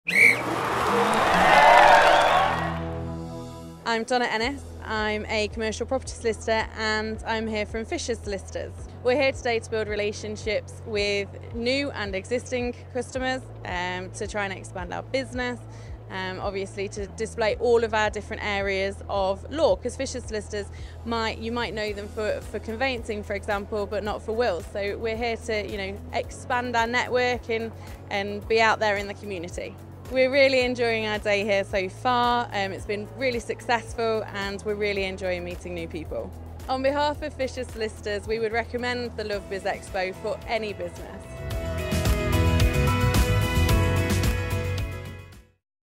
7 LB23 Fishers Solicitors
The chequered flag fell on the Love Business Expo for another year, and this year’s big winners were… everyone who took time out of their day to visit, support or participate in the event.